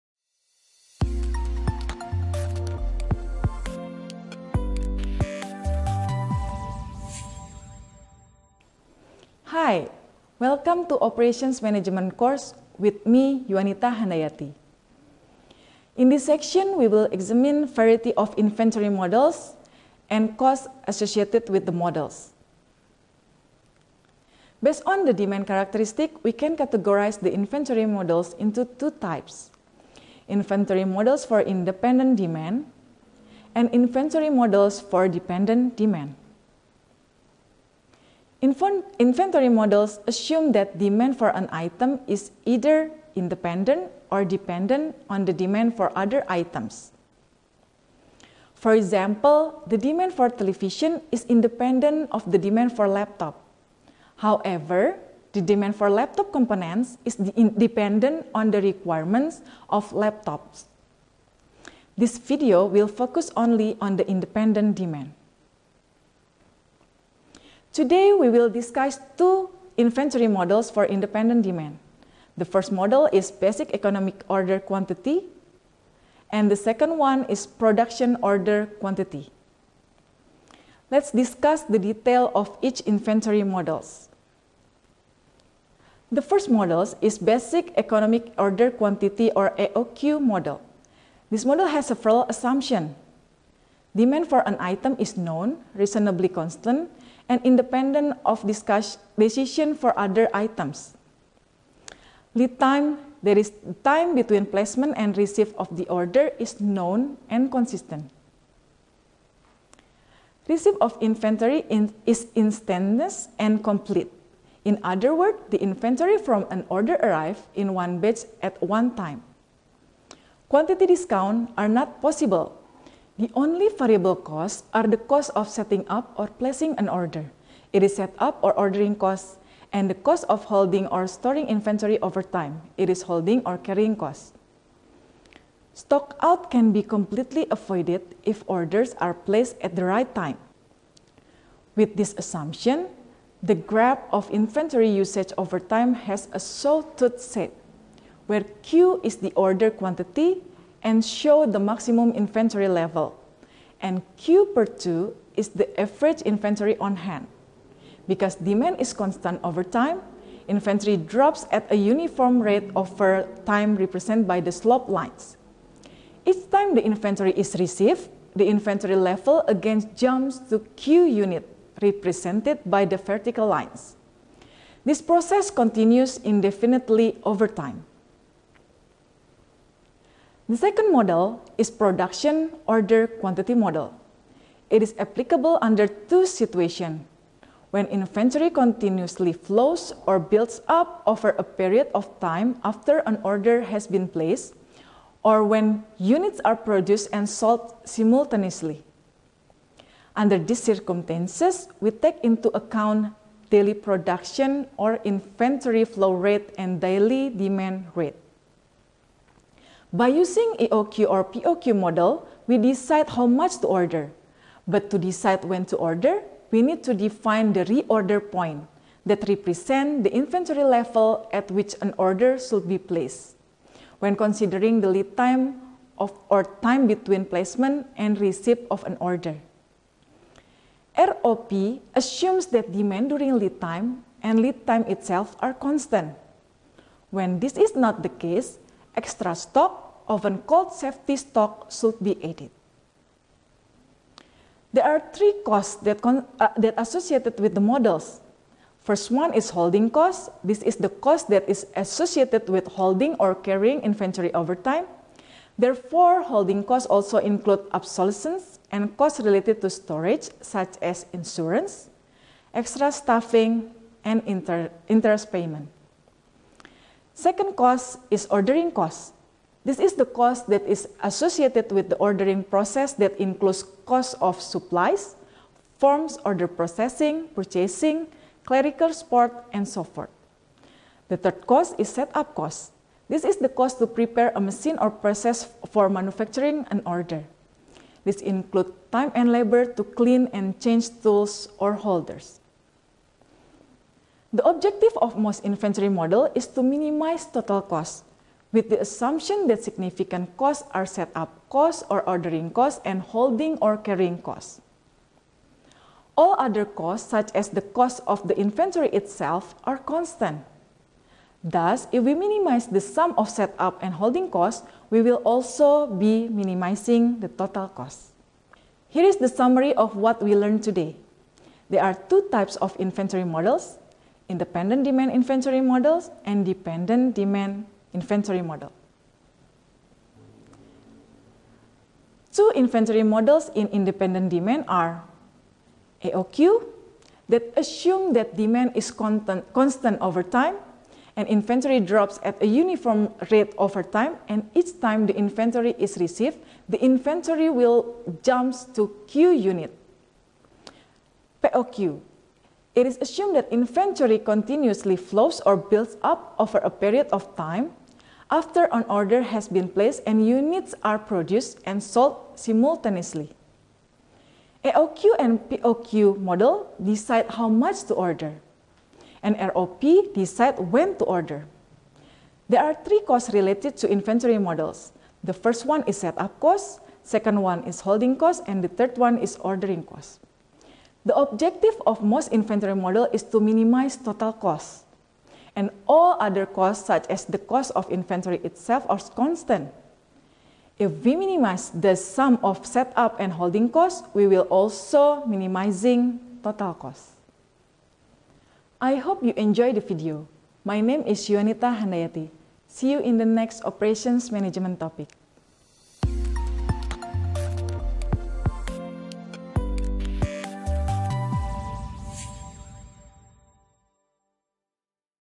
Yuanita Handayati: Inventory Management (Part 3)
In this video, our lecturer, Yuanita Handayati, invites you to examine variety of inventory management models based on demand characteristics. Did you know there are inventory models for dependent demand and independent demand? If you want to learn more about, let’s check out the video!